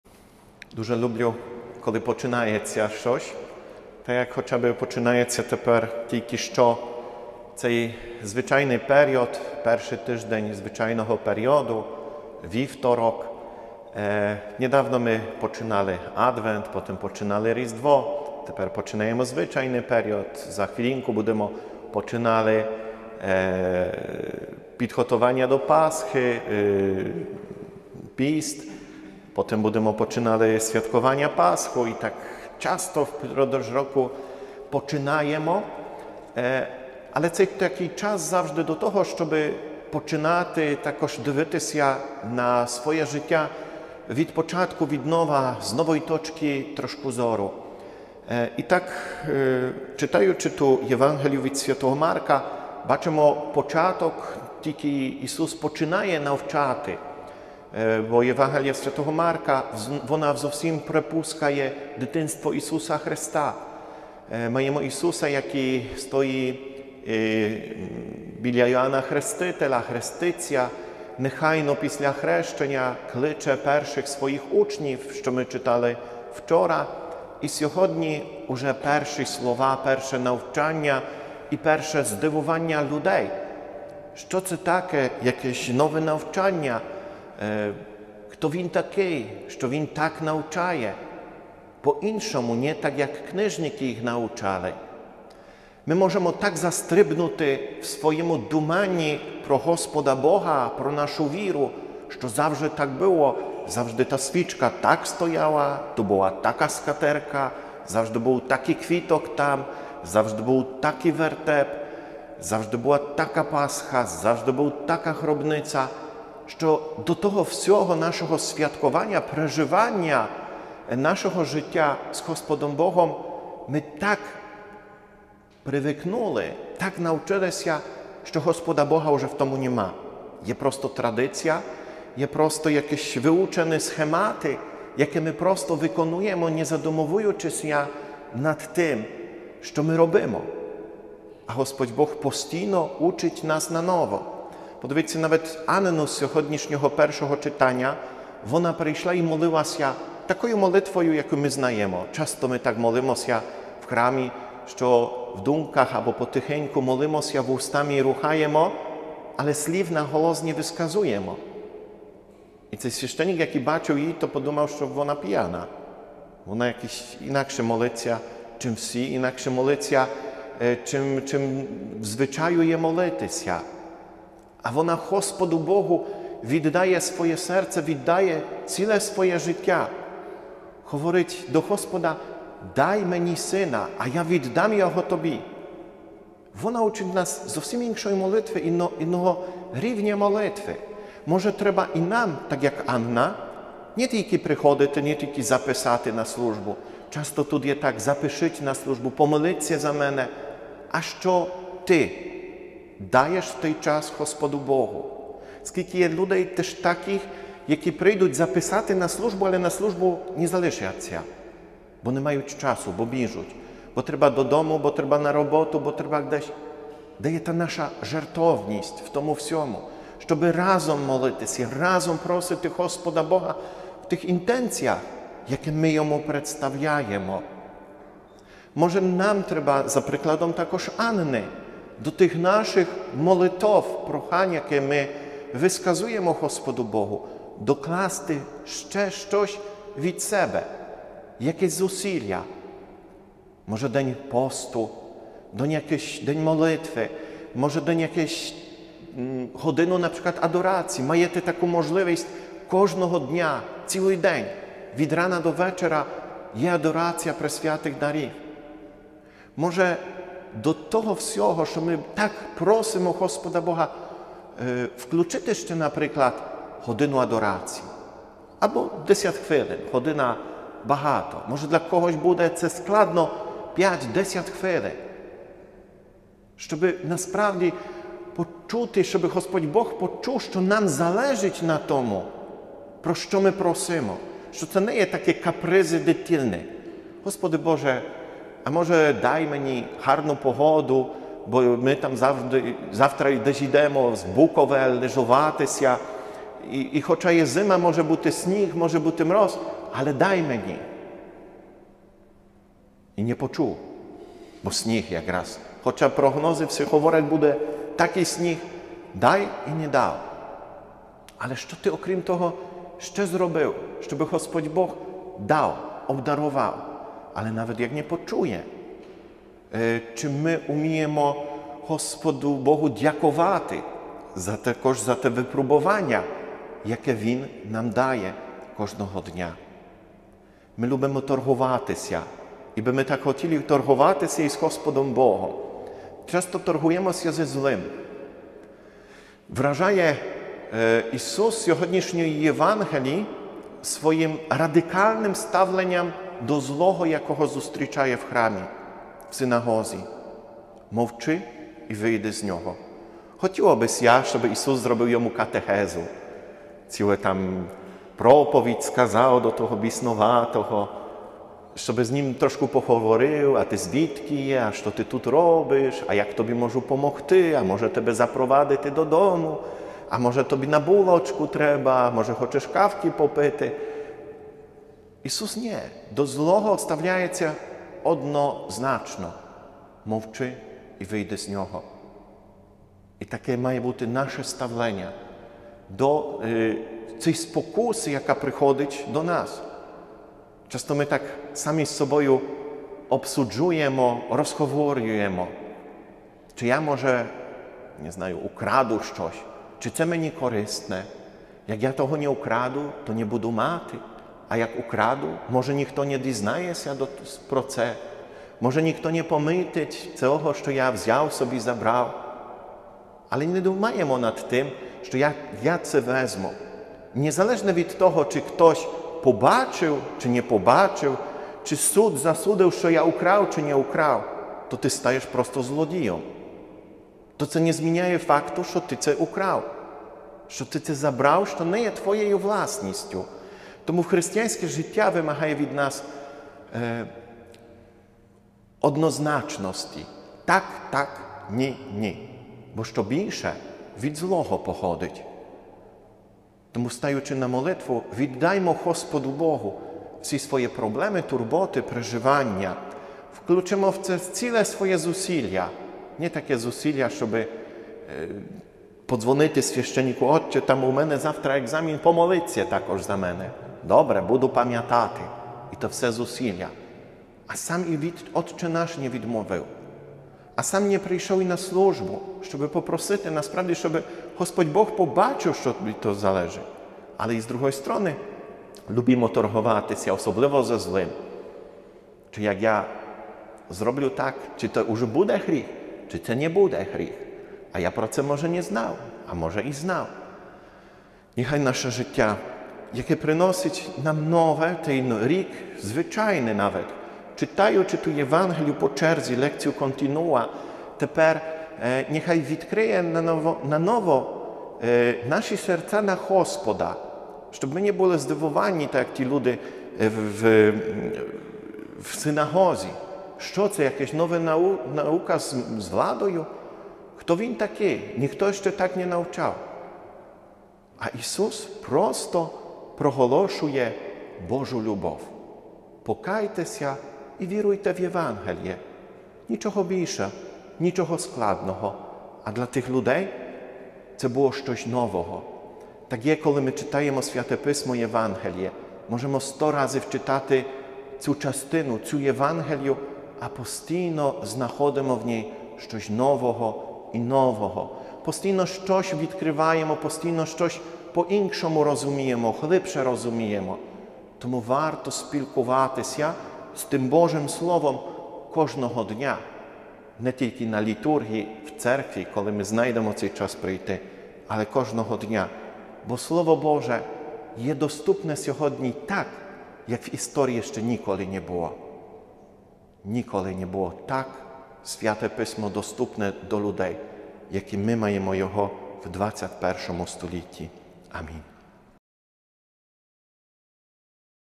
Чи не загубили ми Бога у «традиціях» ? Торги зі злом та брак зусиль Проповідь о. Томаш Коза Sds
Чи не загубили ми Бога у «традиціях» ? Торги зі злом та брак зусиль
Проповідь о. Томаш Коза Sds Tomasz Paweł Koza Sds

🔹Часто ми щось починаємо: і це - шанс заново подивитись на своє життя
У Євангелії чуємо про початок навчання Ісуса, перше здивування людей. Він навчає не так - не так як звикли, не як книжники і фарисеї

🔹Чи не звикли ми так до всіх святкувань, що Господа Бога у них уже нема: є традиція, завчені схеми, а Бога там уже нема?

🔹Господь завжди навчає нас наново

🔹Як ми звертаємось до Нього у молитві? Чи лиш просимо: «запишіть на Службу», «помоліться за мене», і … навіть не йдемо на ту Службу, і чекаємо.. а наші зусилля? А що ТИ даєш у такий час Господу?

Де є наша жертовність? Готовність разом молитися?

Про що ми просимо у молитві? Чи не є це дитячими забаганками: «дай те , дай інше»?

🔹Чи вміємо ми дякувати Господу?

🔹Ми любимо торгуватися. Часто торгуємось і зі злим
Ісус у сьогоднішньому Євангелії вражає радикальним ставленням до злого: замовкни і вийди! Ісус не робив йому катехезу, не пригощав кавою,  не був толерантним - Ісус радикальний : так - так, ні-ні!
А чому ми торгуємося зі злим, закриваємо очі на гріх? Дозволяємо собі «трошки згрішити»: може трохи поосуджую, може трохи собі візьму, може десь закрию на щось очі…?

🤍Нехай наші серця відкриються наново на Господа. Ісус просто проголошує Божу любов: «Покайтеся і віруйте в Євангеліє». Стільки можливостей для вивчення Божого Слова, як у 21 столітті в історії ще не було

ЄвангелієМк 1, 21-28
† Читання святого Євангелія від Марка
В місті Капернаум, в суботу, увійшовши в синагогу, Ісус почав навчати. І дивувалися з Його вчення, тому що Він навчав їх як Той, хто має владу, а не як книжники.І якраз був у їхній синагозі один чоловік, одержимий нечистим духом, і він закричав, кажучи: «Що Тобі до нас, Ісусе Назарянине? Чи Ти прийшов вигубити нас? Знаю, хто Ти є — Святий Божий!»Та Ісус скартав йому, кажучи: «Замовкни і вийди з нього!»Тоді затряс ним нечистий дух і, закричавши гучним голосом, вийшов з нього. І жахнулися всі, так що запитували одне одного, говорячи: «Що це? Нове вчення з владою! Навіть нечистим духам наказує, і вони слухають Його!»І враз розійшлася чутка про Нього всюди, по всій Галілейській околиці.